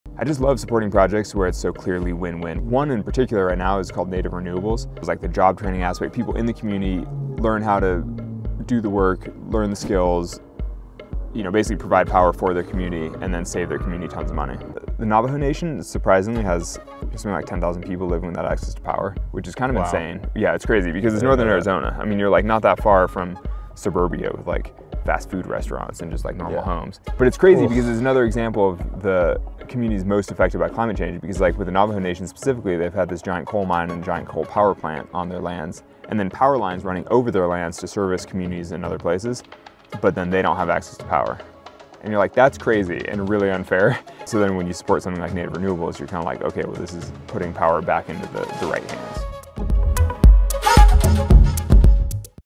Famed climber Alex Honnold shines light on 'unfair' conditions faced by large Indigenous community
The Honnold Foundation is partnering with nonprofit organization Native Renewables to provide access to sustainable forms of electricity to members of the Navajo Nation in Arizona.